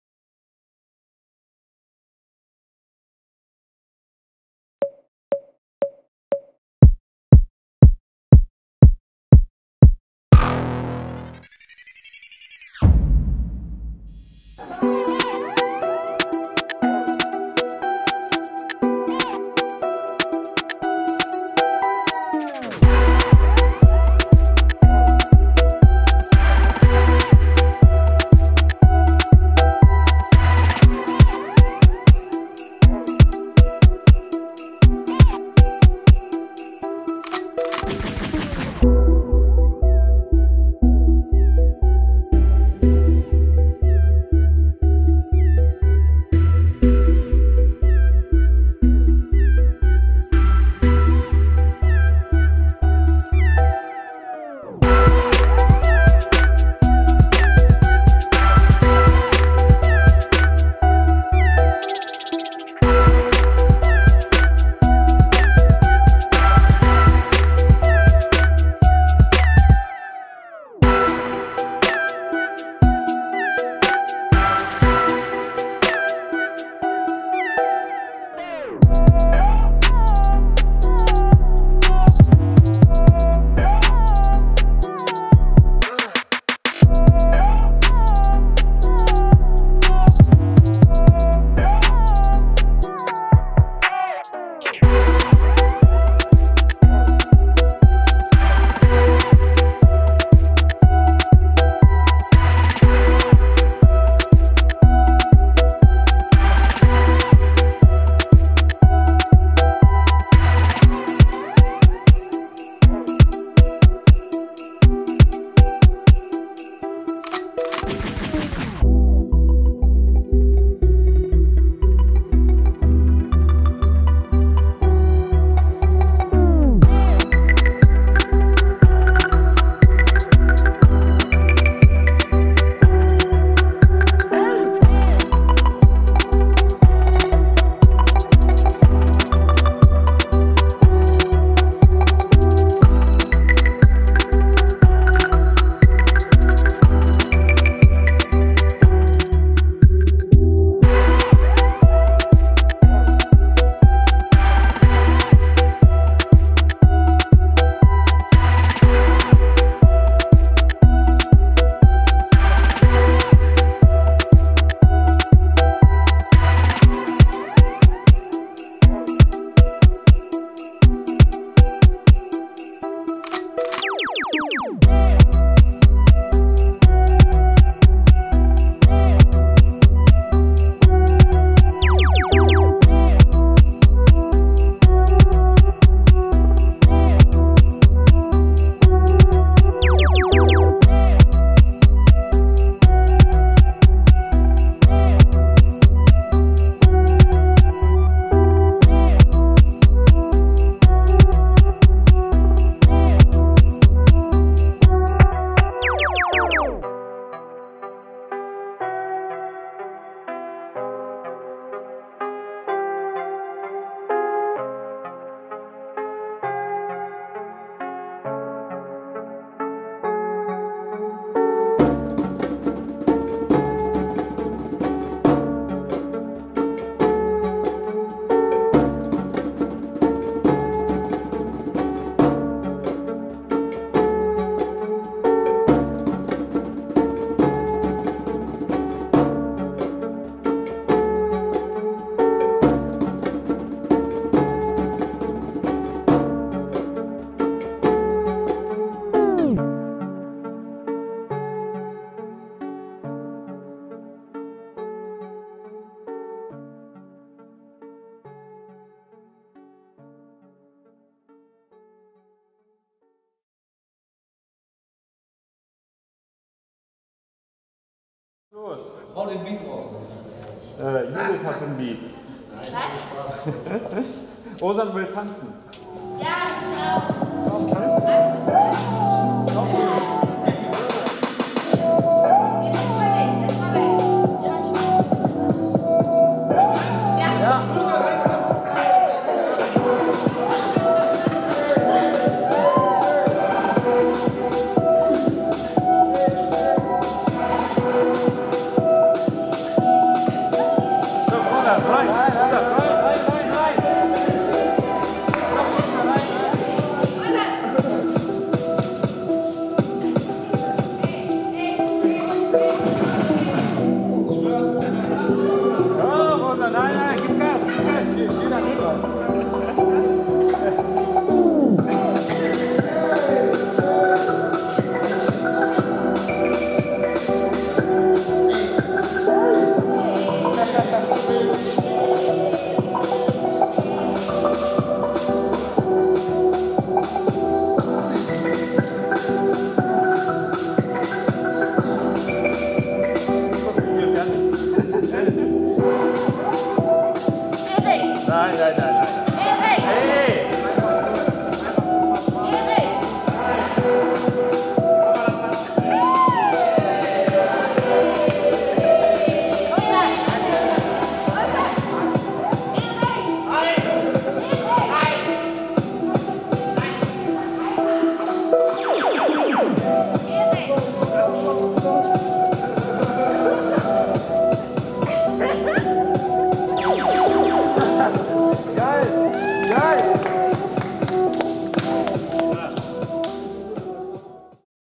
DNC - ein Tanzvideo
K-town Productions von Cactus Junges Theater präsentiert

Im Winter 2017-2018 ist unter dem Label K-town Productions ein Tanzvideo entstanden. Gedreht wurde in der Tiefgarage eines Hochhauses in der Kinderhauser Schleife. Dafür hat der Musiker Valon Labjani für fünf TänzerInnen nach deren Wunsch einen Afrotrap-Tanzbeat produziert. Unter der Leitung von Risper Wangui sind Soli und Gruppenchoreos entwickelt worden - ein 4 min Video, das die Bewegungsfreude und den Tanz feiert.

Tänzer: Michael Yaw Asabere, Voila Kappel, Hashem Hadi, Naso Sabanovic, Risper Wangui
Choreographie: Risper Wangui und Tänzer
Regie: Judith Suermann
Kamera: Erich Saar
Musikproduzent: Felix van Beuse

Eine Produktion von Cactus Junges Theater in Kooperation mit dem Jugendtheater-Werkstatt e.V. und dem Internationalen Kulturverein Atrium e.V.
Gefördert durch: die AKTION MENSCH